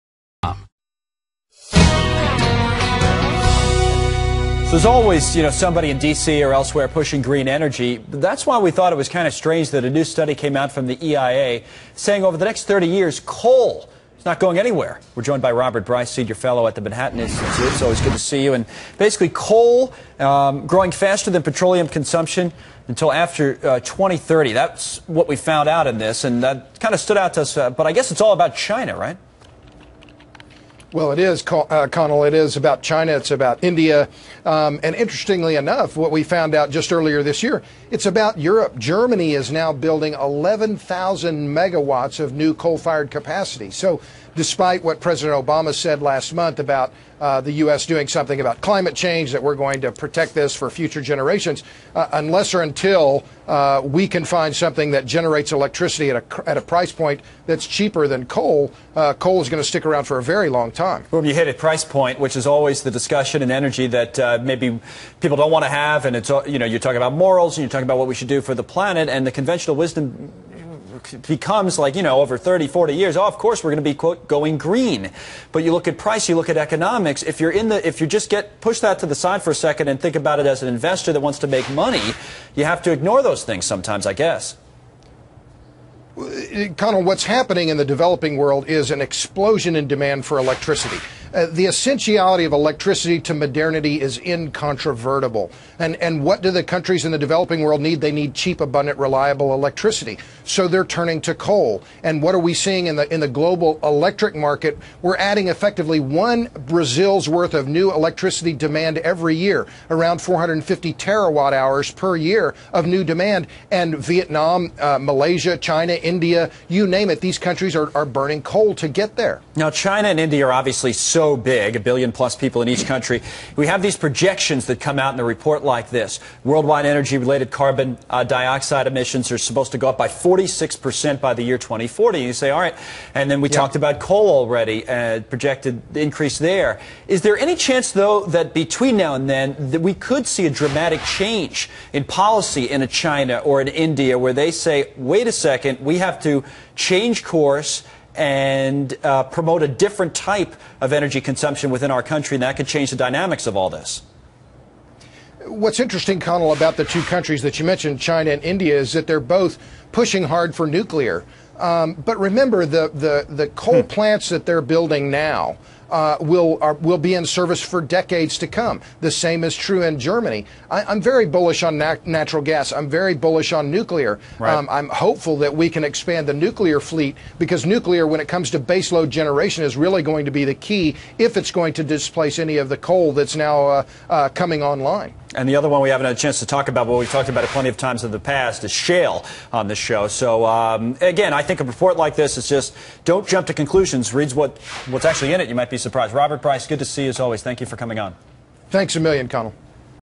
Coal Consumption Expected to Grow
Manhattan Institute senior fellow Robert Bryce on coal remaining a top source of power.